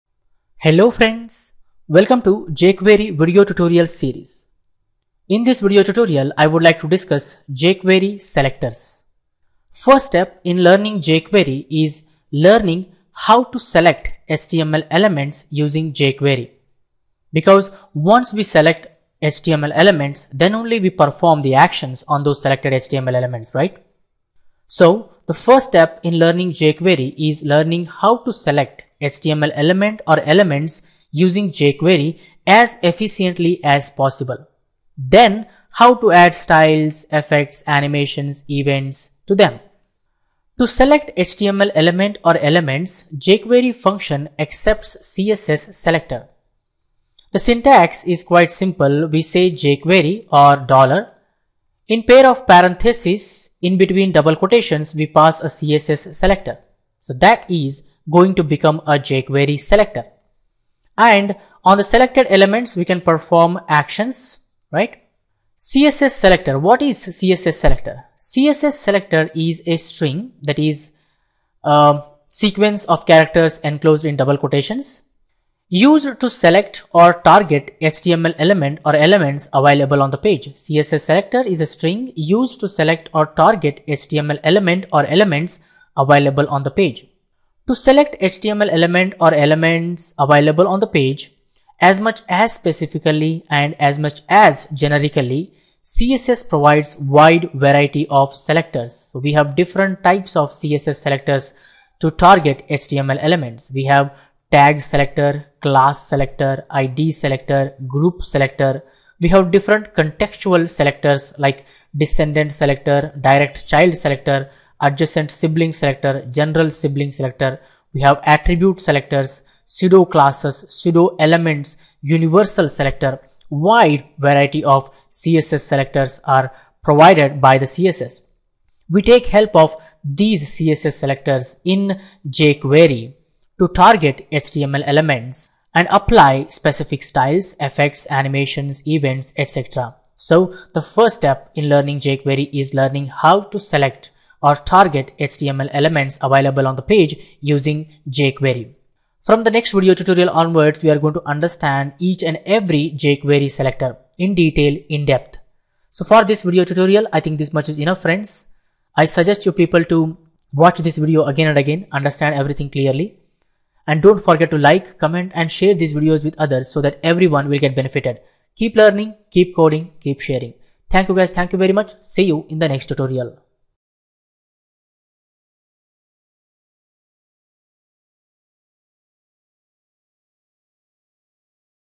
jQuery Selectors Tutorial - jQuery Tutorial 11 🚀
► Get introduced to jQuery selectors in this beginner-friendly tutorial! Learn how to select, manipulate, and interact with HTML elements efficiently using jQuery’s powerful selector system.